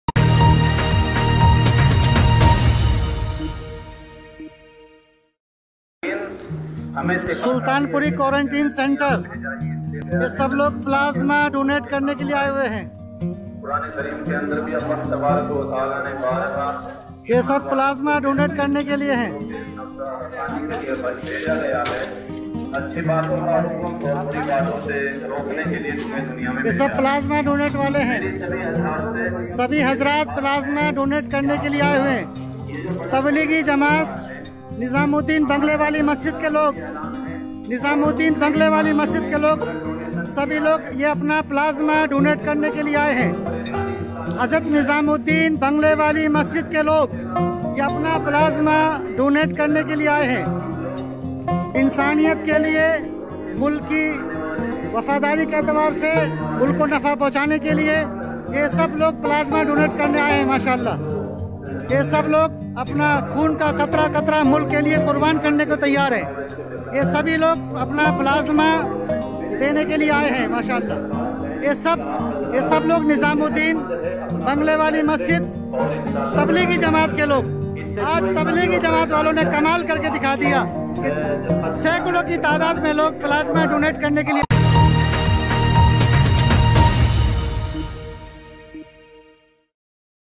जमात के रक्तदान से बचाए जा रहे हैं कोरोना के मरीज़?
तब्लीग़ी जमात पर लगनेवाले इलज़ाम और उसकी वास्तविकता, अब सच्चाई सबके सामने आ चुकी है। जिनपर कोरोना फैलाने का इल्ज़ाम था, आज उन्हें कोरोना से बचानेवाला कहने लगे हैं।ये कैसे हुआ?